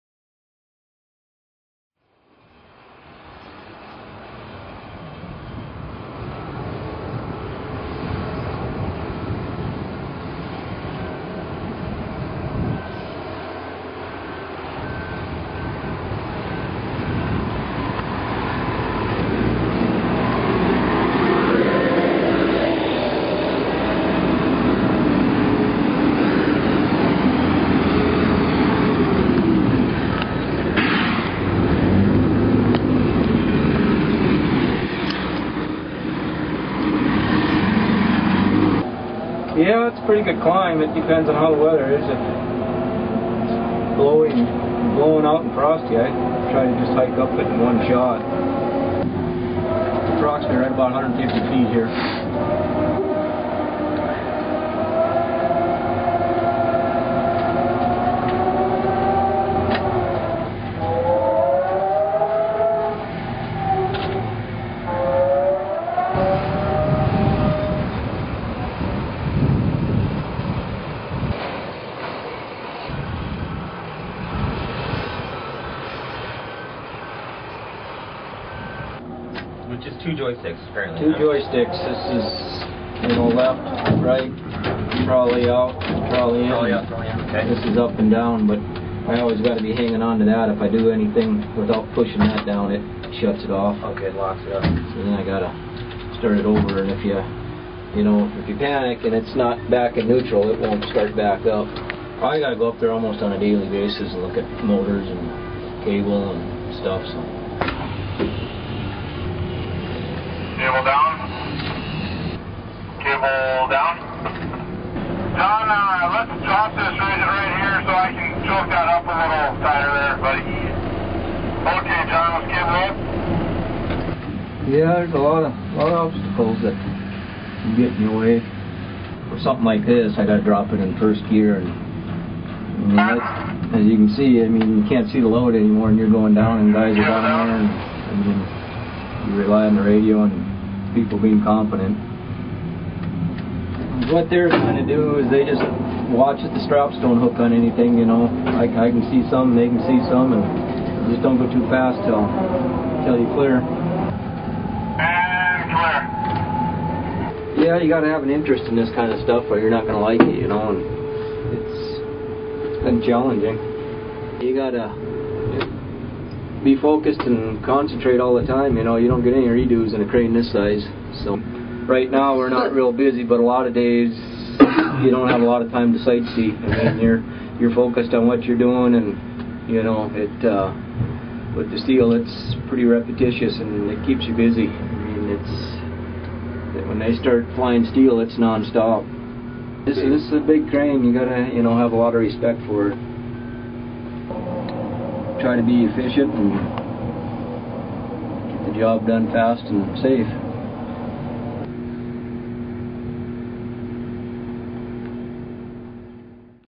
The Crane: A Room with a View
A photographer views the site of the new State Fund building from the perspective of crane operator John Nuebel